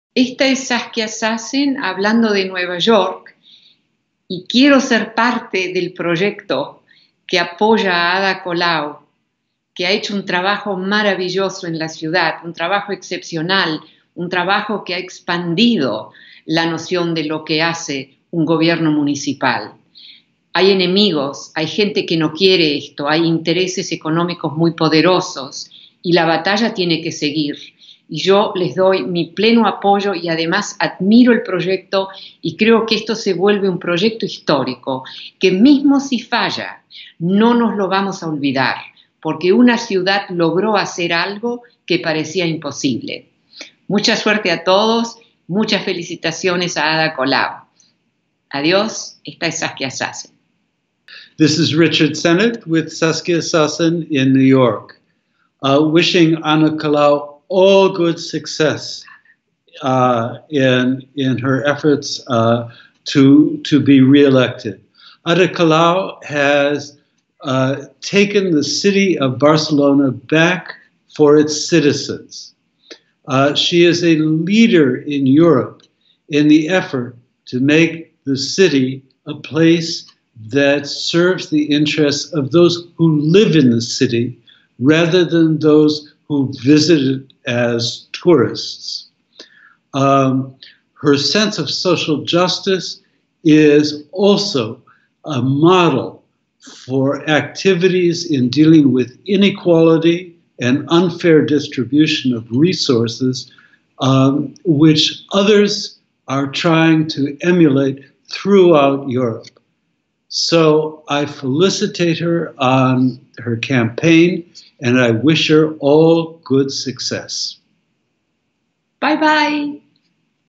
Saskia Sassen y Richard Sennet apoyan a Ada Colau
Vídeo de apoyo de Saskia Sassen y Richard Sennet. Comparte tu apoyo con ConAdaSí

Mensaje de Saskia:
Soy Saskia Sassen desde Nueva York y quiero ser parte del proyecto que apoya a Ada Colau. Ella ha hecho un trabajo maravilloso en la ciudad, un trabajo excepcional, que ha expandido la noción de lo que hace un gobierno municipal. Hay enemigos, gente que no quiere esto, hay intereses económicos muy poderosos, y la batalla tiene que seguir. Yo les doy mi pleno apoyo y, además, admiro el proyecto. Creo que se vuelve un proyecto histórico, aunque fallara no lo olvidaremos. Porque una ciudad logró hacer algo que parecía imposible. Mucha suerte a todos y felicitaciones a Ada Colau, de Saskia Sassen.

Mensaje de Richard Sennet:
This is Richard Sennett with Saskia Sassen in New York wishing Ada Colau all good success in her efforts to to be reelected Ada COlau has taken the city of Barcelona back for its citizens. She is a leader in Europe in the effort to make the city a place that serves the
interests of those who live in the city rather than those who visited as tourists. Her sense of social justice is also a model for activities in dealing with inequality and unfair distribution of resources which others are trying to emulate throughout Europe.
So I felicitate her on her campaign and I wish her all good success.

Tranducción del mensaje de Richard Sennet:

Soy Richard Sennet, estoy con Saskia Sassen en New York. Le deseamos a Ada Colau todo el éxito del mundo en su campaña para ser reelegida. Ada Colau ha devuelto la ciudad de Barcelona a sus ciudadanos. Es líder en Europa en la labor de transformar la ciudad en un sitio que sirva a los intereses de los ciudadanos que viven en ella y no de los que la visitan como turistas. Su sentido de la justicia social es también un modelo para movimientos y actividades que tratan la desigualdad y la distribución injusta de recursos que están imitando en toda Europa.
Por ello le felicito por su campaña y le deseo todo el éxito del mundo.